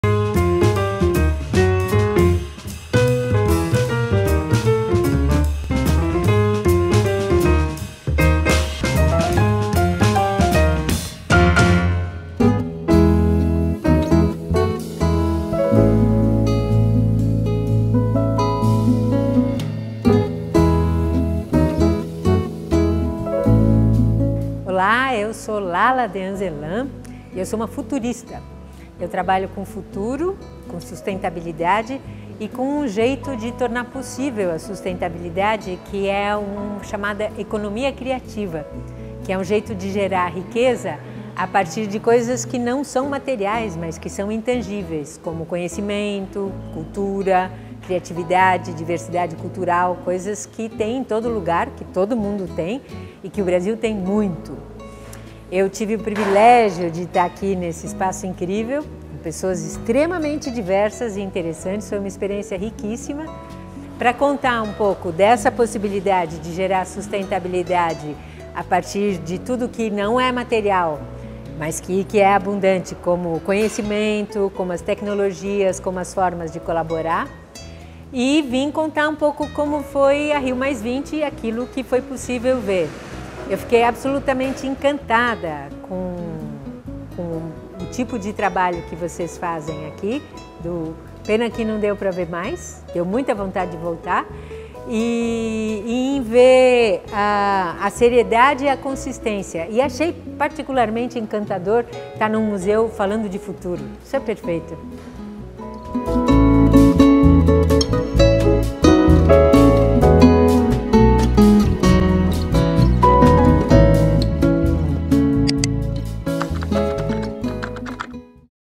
Lala Deheinzelin - Desejável Mundo Novo
Porque não vivermos em mundo sustentável? Essa foi a grande questão norteadora da palestra desta quinta-feira, 28 de junho, no Língua Afiada, que trouxe a assessora internacional e especialista mundial em Economia Criativa, Sustentabilidade e Futuros, Lala Deheinzelin, para falar dos desdobramentos da Rio+20 e para lançar, em Belo Horizonte, seu livro Desejável Mundo Novo. Leia mais e assista à palestra completa